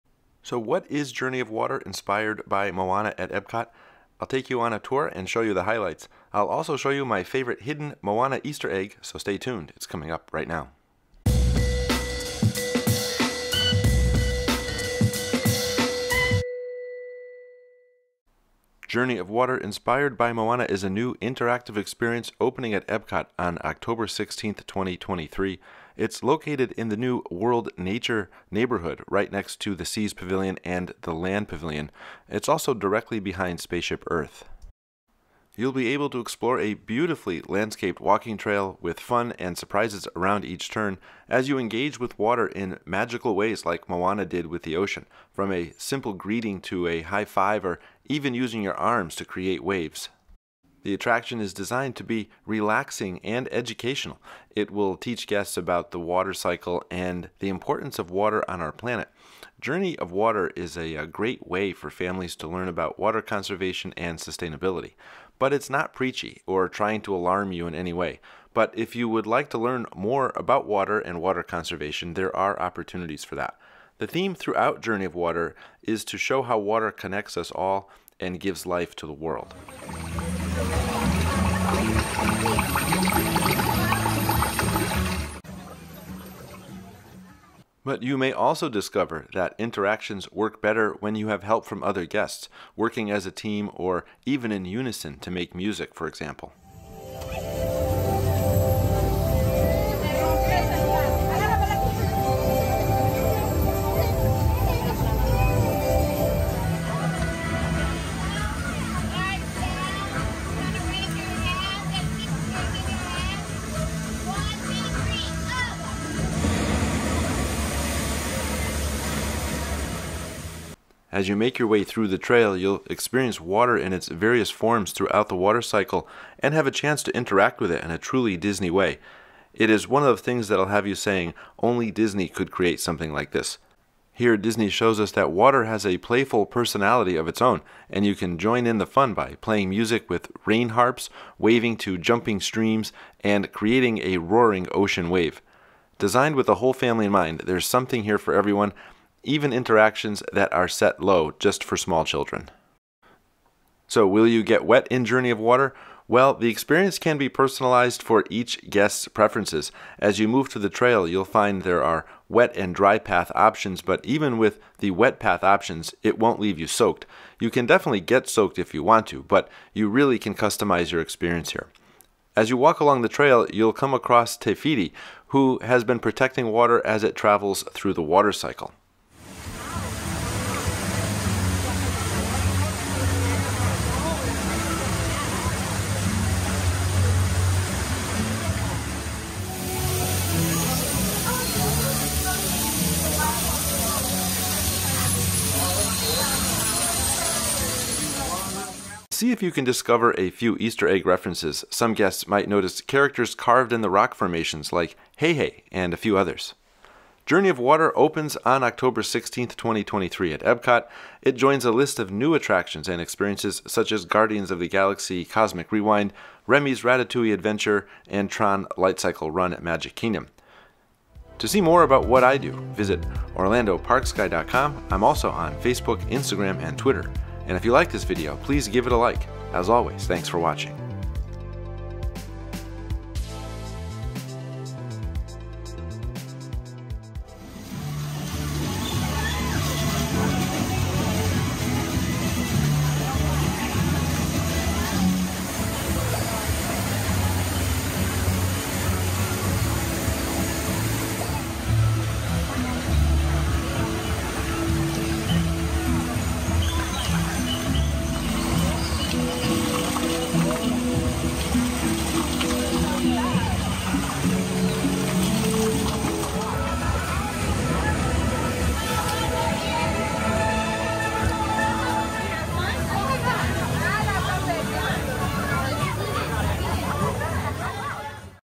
Go inside Journey of Water at Epcot (Walkthrough)
Walkthrough and highlights of Journey of Water Inspired by Moana - Epcot's newest attraction opening this fall. "Journey of Water" feels like a tropical oasis in the middle of Epcot. It ties in nicely with The Land and The Seas pavilions.

Journey of Water, Inspired by Moana is a new, interactive experience opening at Epcot on October 16, 2023. It’s located in the World Nature neighborhood, right next to The Seas Pavilion and The Land Pavilion, directly behind Spaceship Earth.

You’ll be able to explore a beautifully landscaped walking trail with fun and surprises around each turn, as you engage with water in magical ways like Moana did with the ocean – from a simple greeting to a high five or even using your arms to create waves.

The attraction is designed to be a relaxing and educational experience. It will teach guests about the water cycle and the importance of water on our planet. Journey of Water is a great way for families to learn about water conservation and sustainability.

But it’s not preachy, or trying to alarm you in any way. But if you would like to learn more about water and water conservation, there are opportunities for that.  The theme throughout Journey of Water is to show how water connects us all and gives life to the world.

But you may also discover that the interactions work better when you have help from other guests - working as a team or even in unison to make music.

As you make your way through the trail, you’ll experience water in its various forms throughout the water cycle and have the chance to interact with it in a truly Disney way. It is one of those things that will have you saying, “Only Disney could make something like this.”

Disney shows us that water has a playful personality of its own, and you can join in the fun by playing music with “rain harps,” waving to jumping streams, and creating a roaring ocean wave.

Designed with the whole family in mind, there’s something for everyone, even interactions that are set low for children.

Will you get wet in Jouneny of Water? Well, the experience can be personalized for each guest’s preferences. As you move through the trail, you’ll find wet and dry path options but even the wet path options won’t leave you soaked. You can definitely get soaked if you want to, but you really can customize your experience here.

As you walk along the trail, you’ll come across Te Fiti, who is seen protecting water as it travels through the water cycle.

And see if you can discover a few easter egg references some Guests might notice characters carved in the rock formations like Hei Hei and friends.

Journey of Water opens on October 16, 2023 at Epcot. It joins a list of new attractions and experiences such as Guardians of the Galaxy Cosmic Rewind, Remy’s Ratatouille Adventure, and Tron Lightcycle Run at Magic Kingdom